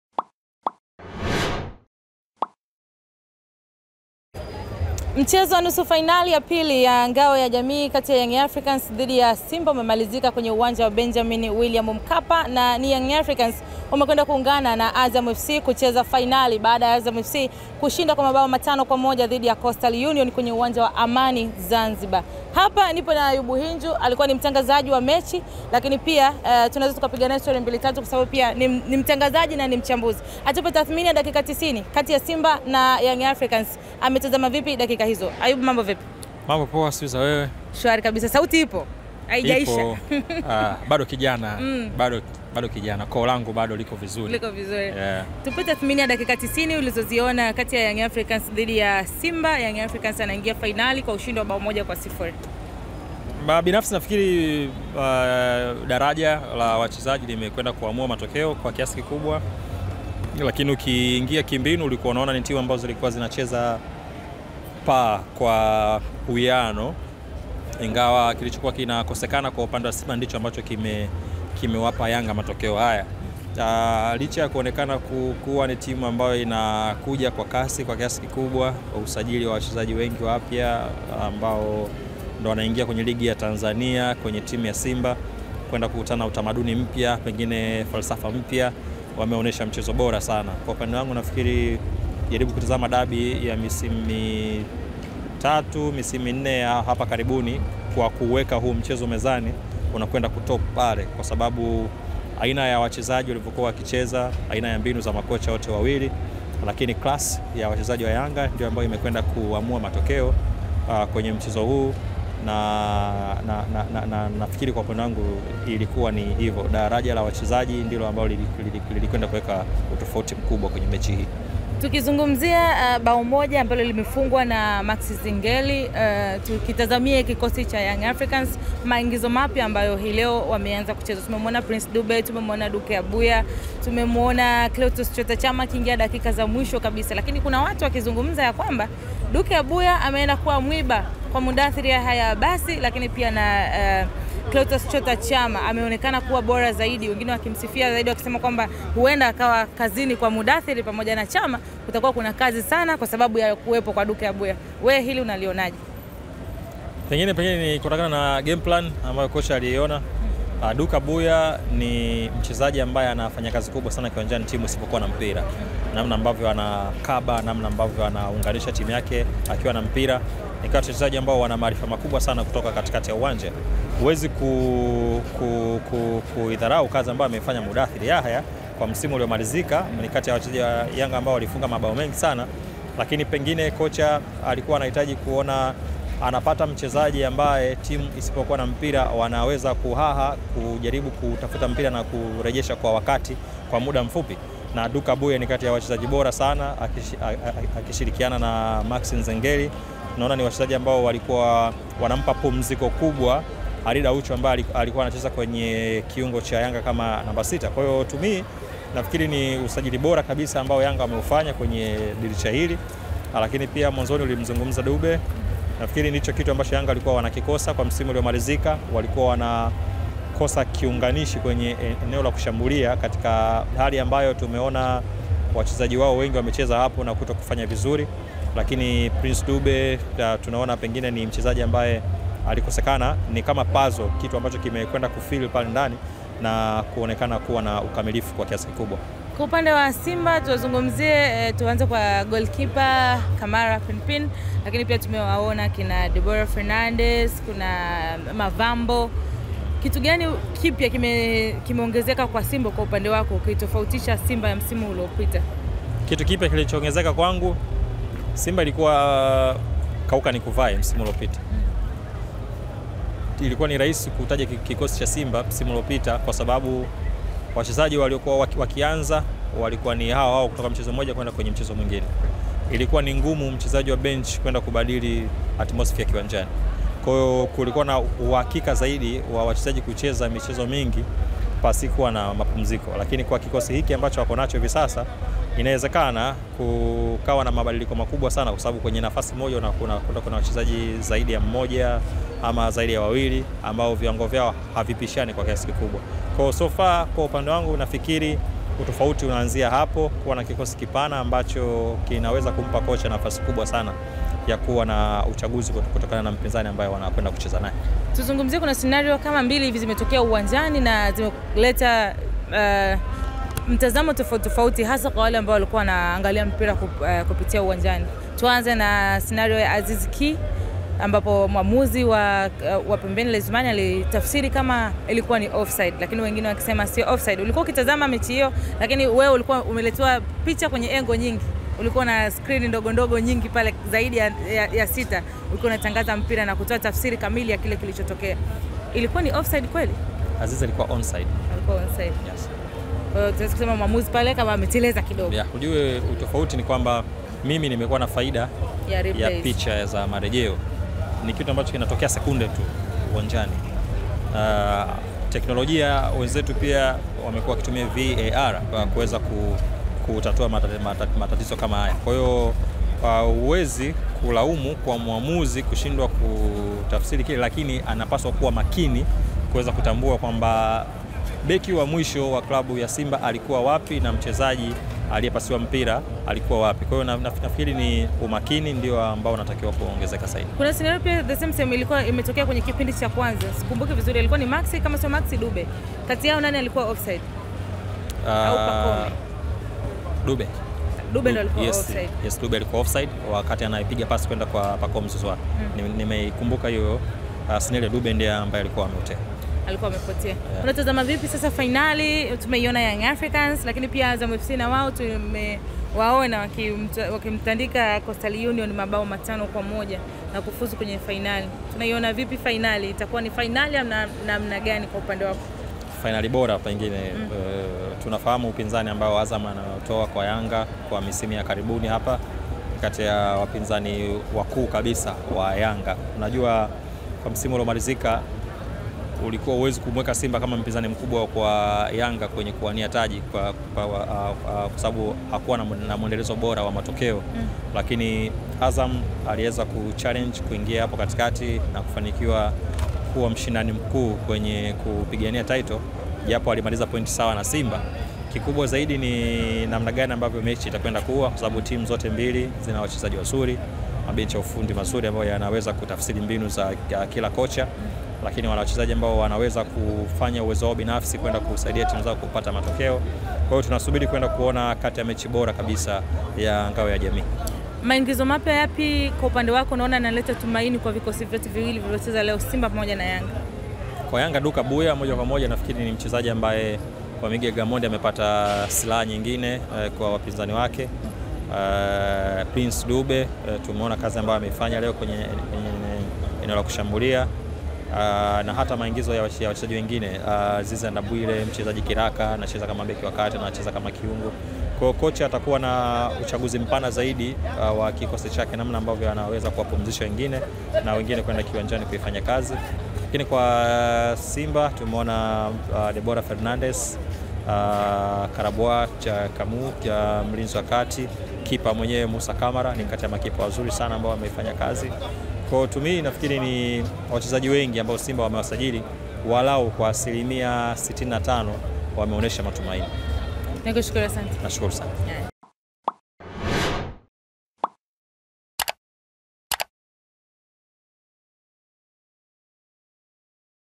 AYUBU HINJO atoa ufafanuzi kuhusu bao la AZIZ KI, ni OFF SIDE ?
Ayoub Hinju atoa ufafanuzi kuhusu bao la Aziz Kİ, Ni off side?